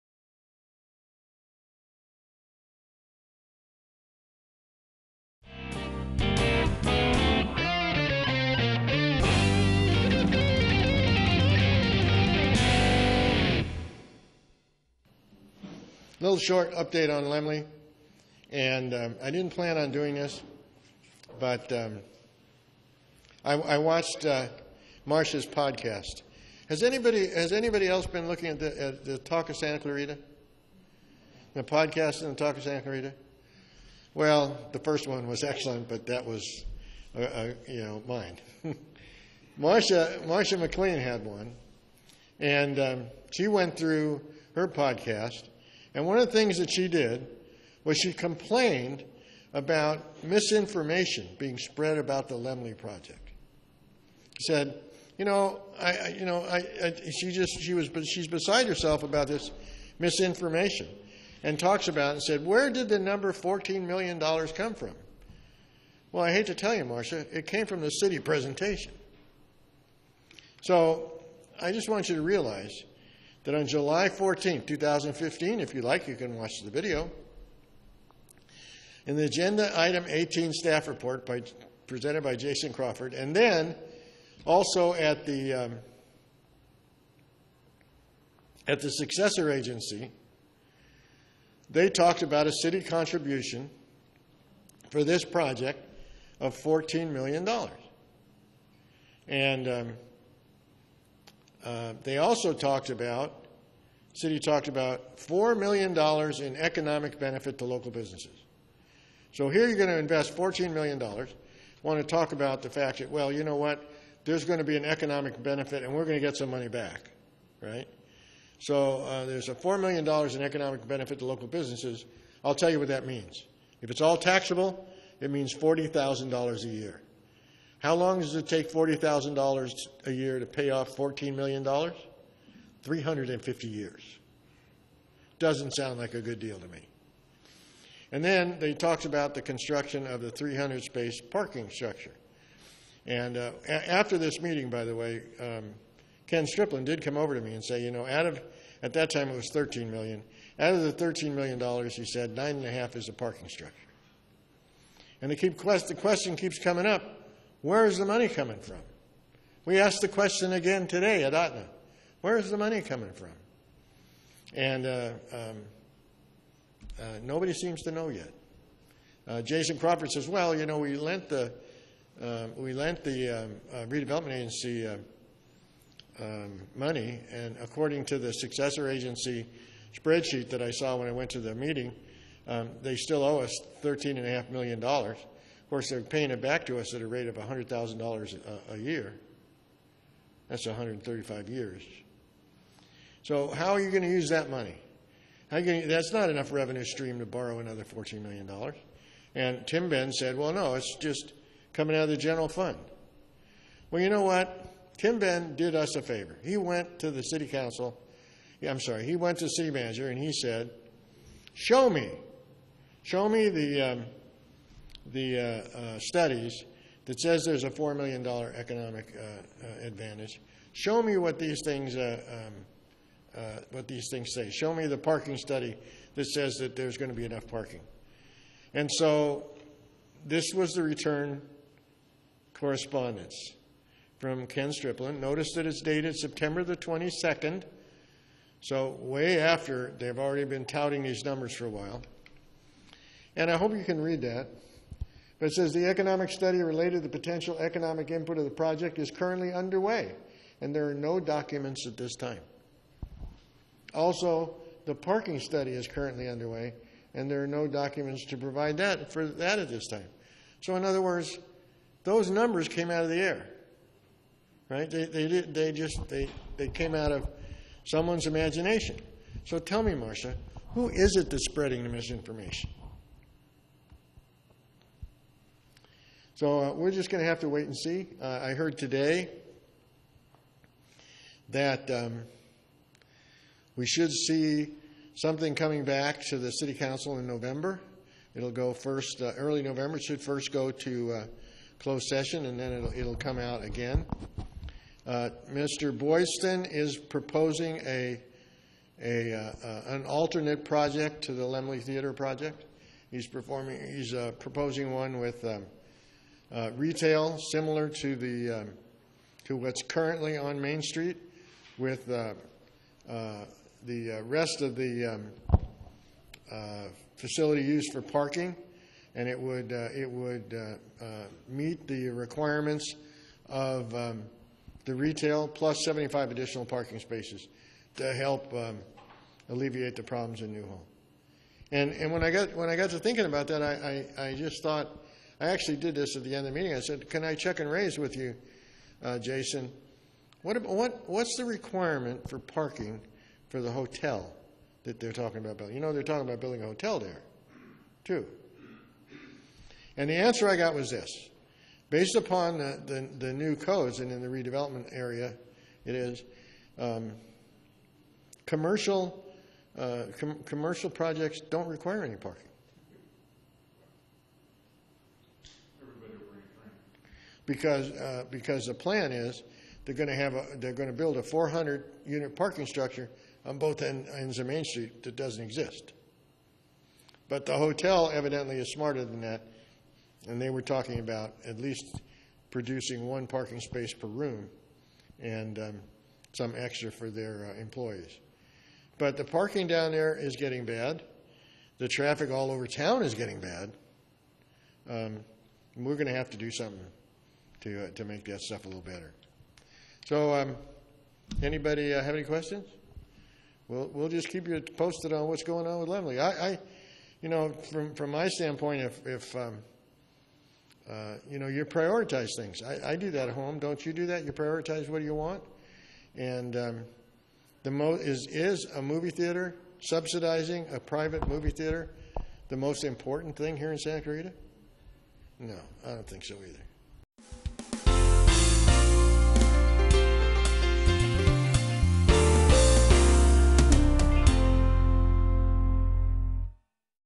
CCAC102115ShortLaemmleUpdateMP
Short Laemmle Theater Project update.  Should the City of Santa Clarita provide this project with a $14,000,000 subsidy??  Presented by Alan Ferdman, Chair CCAC, at the October 21, 2015 Canyon Country Advisory Committee Meeting, held in the Mint Canyon Moose Banquet Room, 18000 Sierra Highway in Canyon Country, 91351